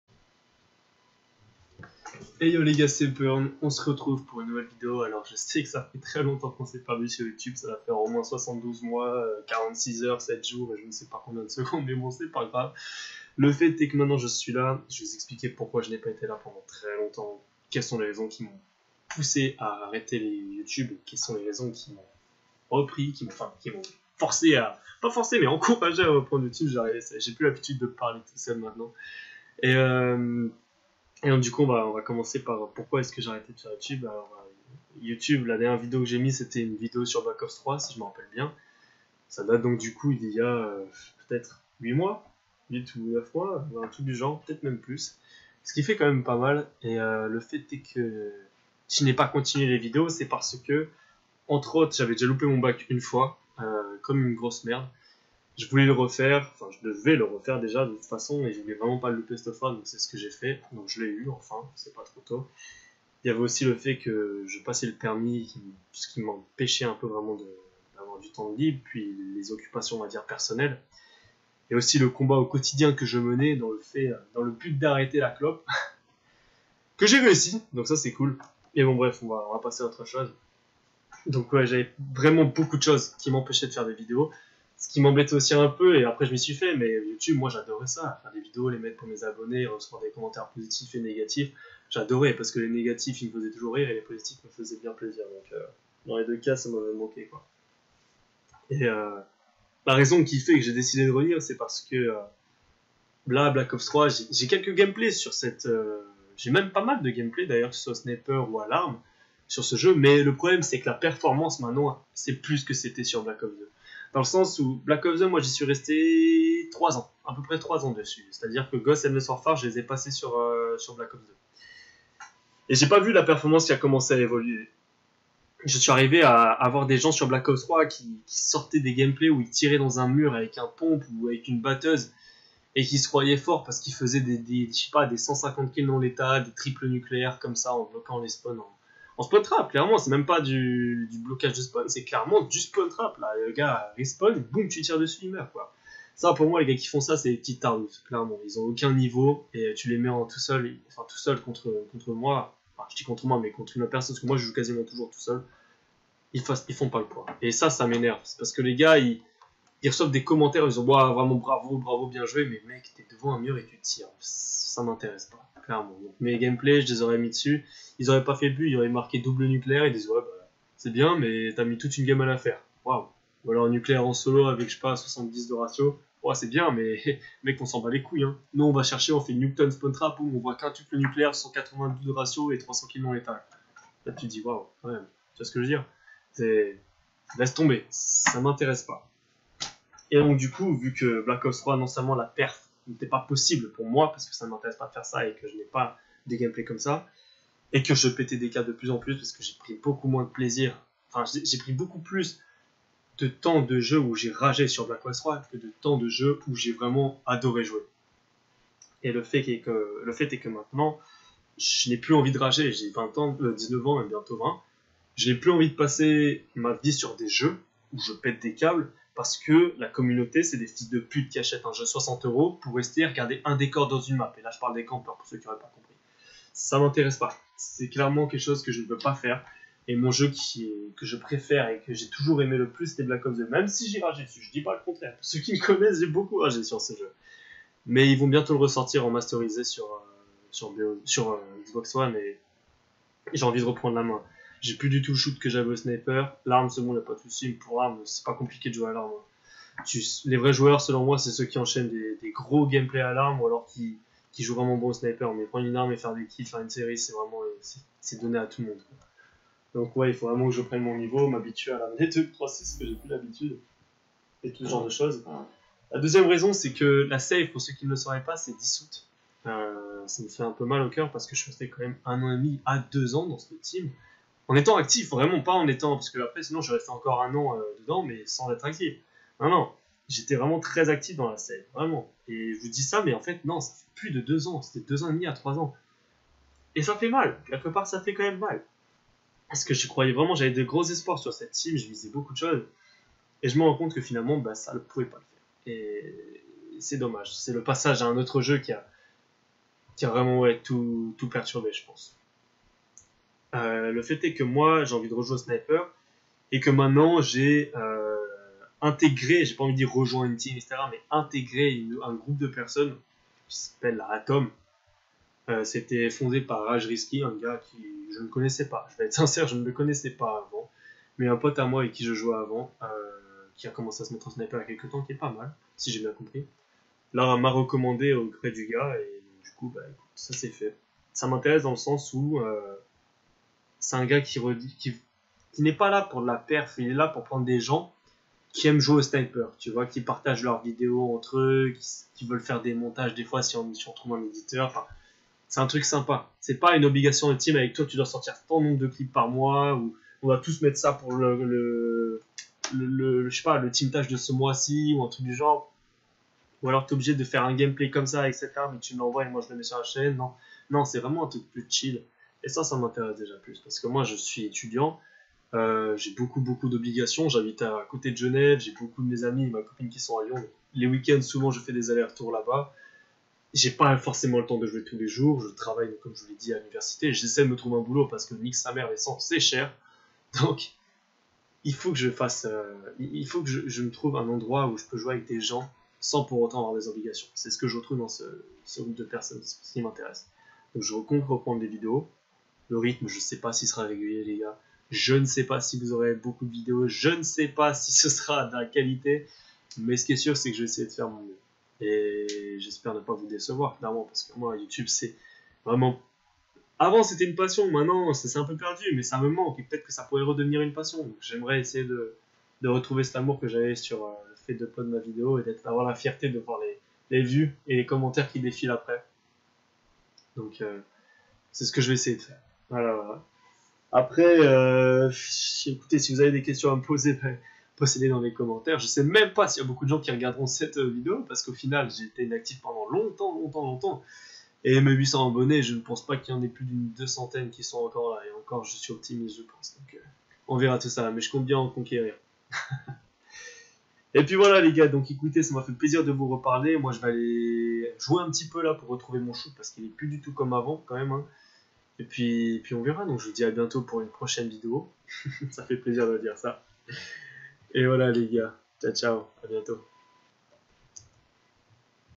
MON RETOUR - ANNONCE ET PROGRAMME A VENIR
Les gars je suis extremement content de revenir dans le monde de youtube je vous explique tout dans la vidéo, j'espère que vous apprécierez et je vous dis a bientôt !